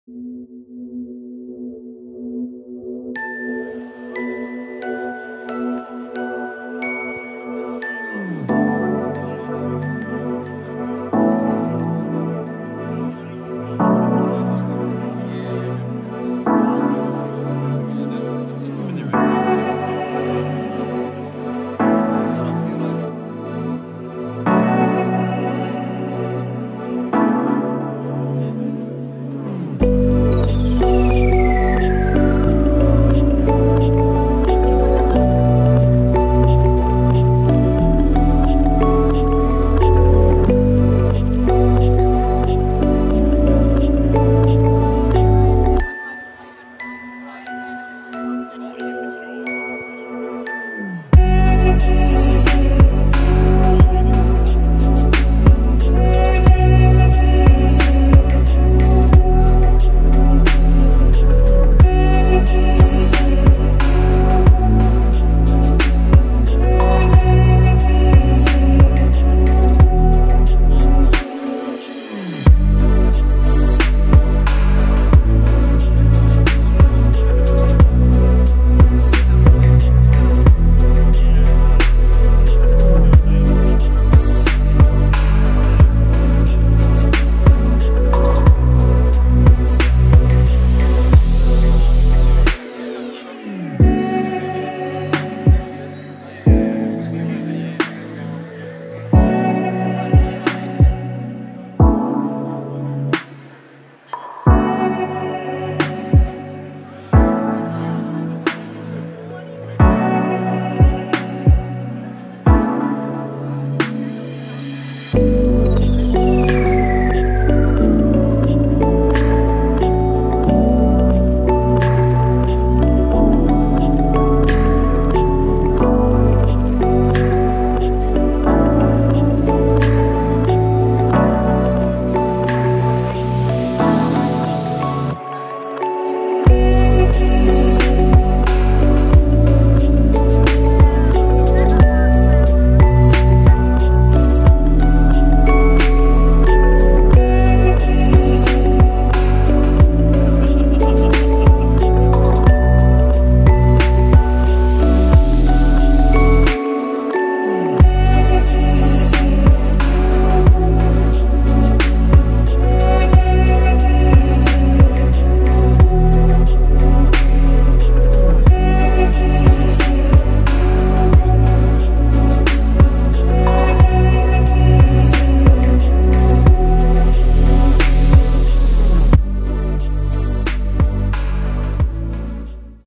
Reefstock Australia 2018 || The Coral Saga
Over the 11th and 12th of August this year we had the First Ever Reefstock Australia hosted by Reef Builders.

This is only a small sample of some of the amazing corals that were on display (and for sale) at this show! This is only just the tip of the iceberg to show you some of the corals that were at the show!

*I Hope to have videos out at least once every two weeks but I have no set schedule.